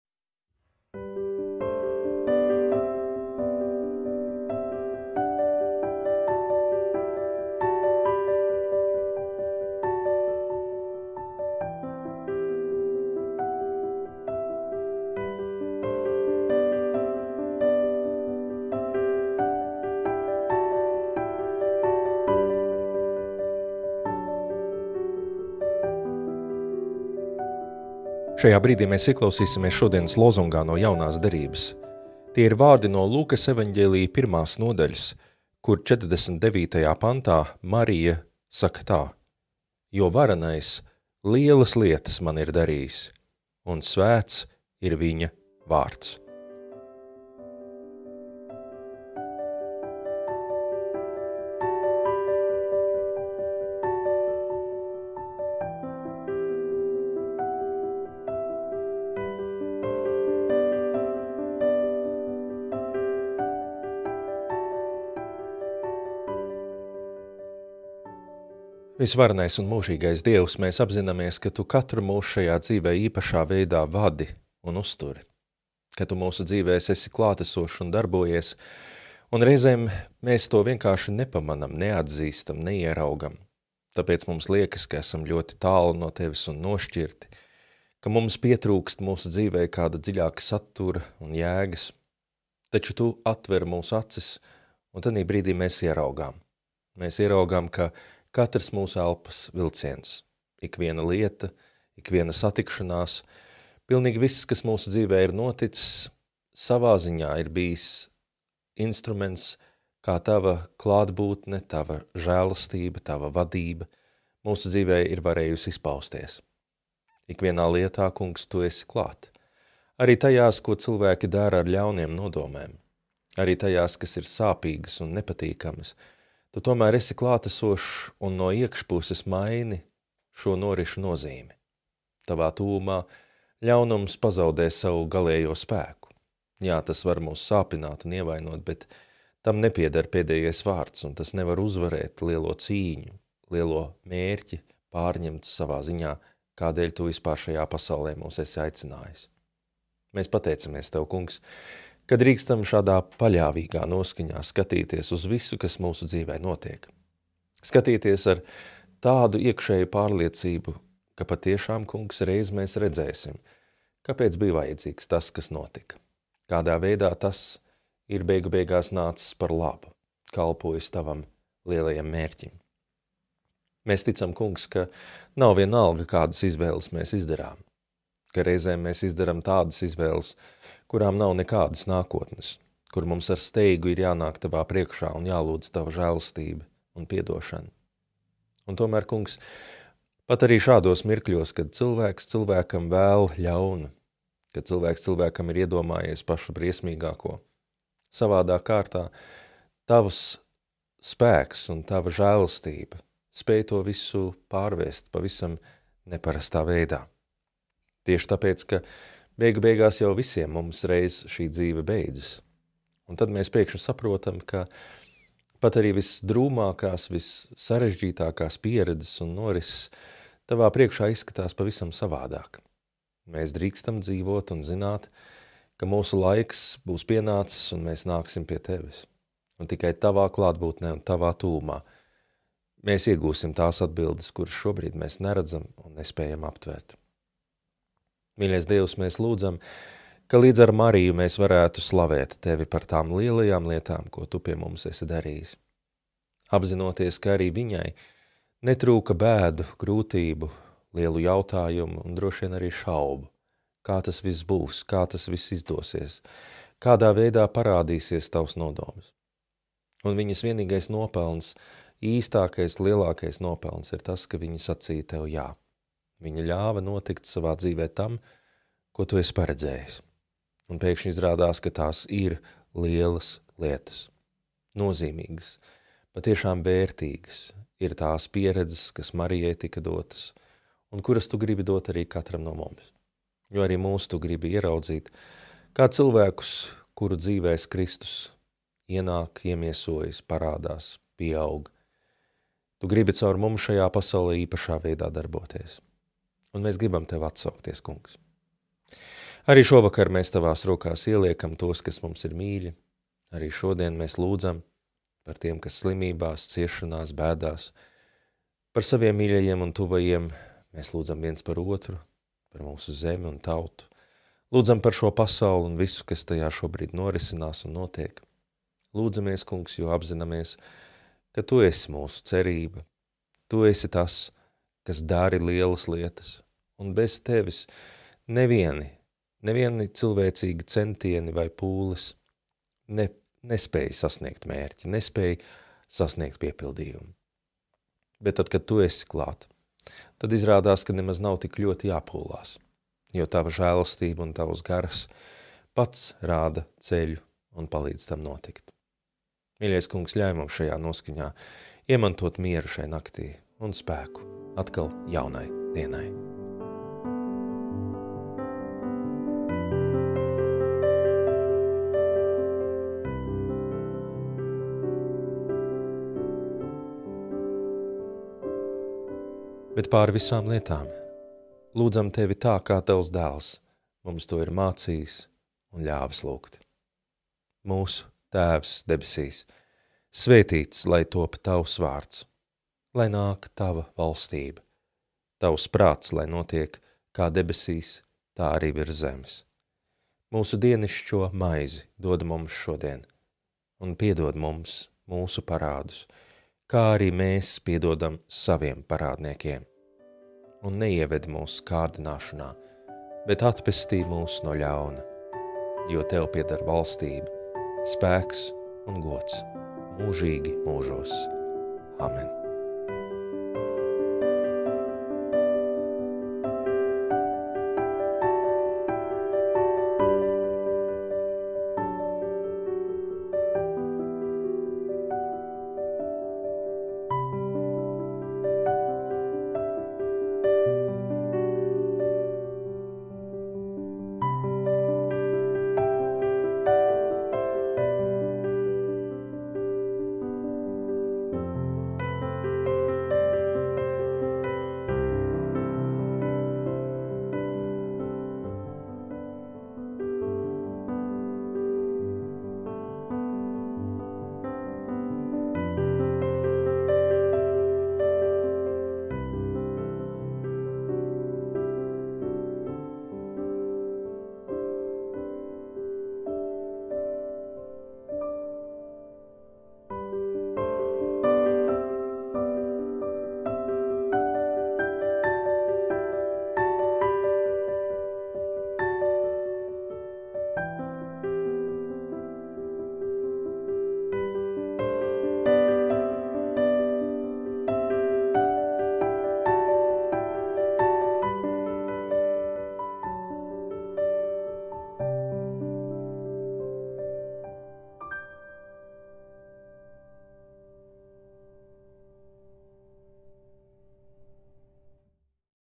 ELIZEJAS VAKARA LŪGŠANA – 13.jūnijs
Lūgšanas iedvesmai šīs dienas lozungs no Jaunās Derības:

Jo Varenais lielas lietas man ir darījis, un svēts ir Viņa Vārds. (Lk 1:49)